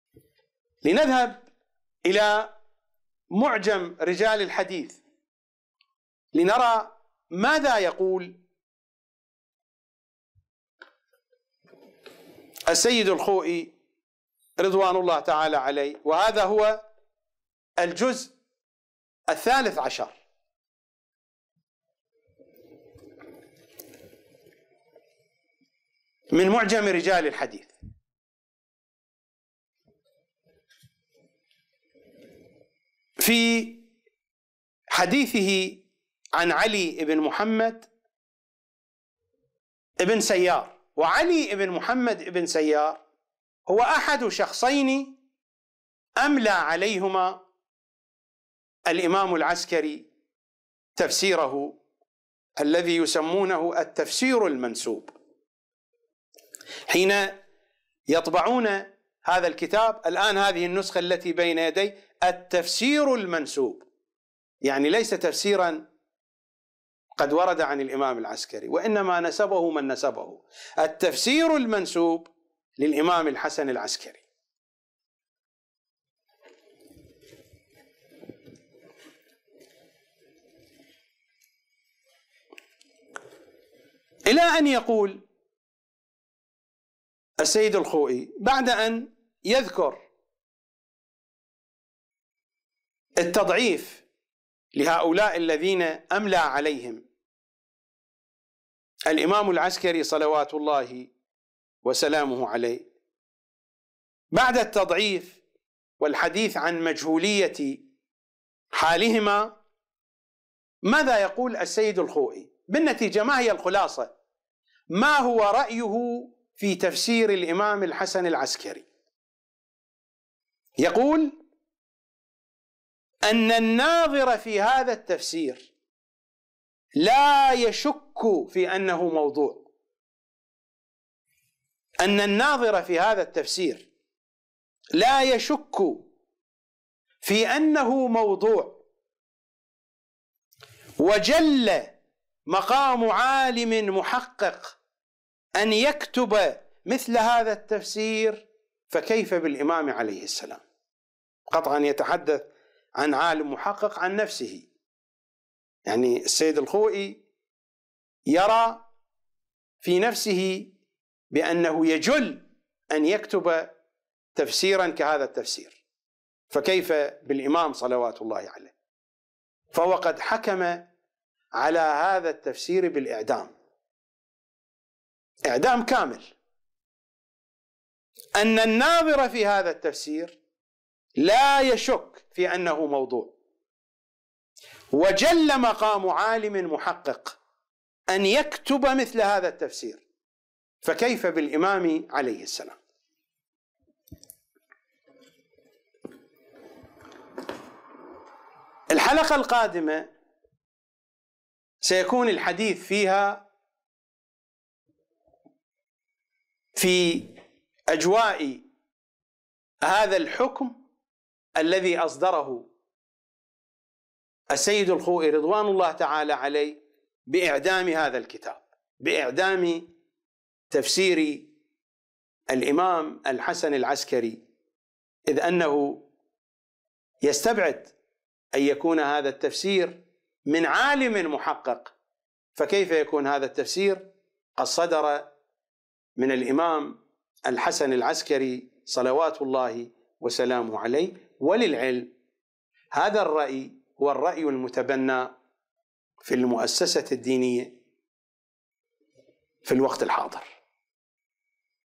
السيد الخوئي يحكم بالاعدام على تفسير الامام العسكري عليه السلام - الشيخ الغزي
الكتاب الصامت ح 2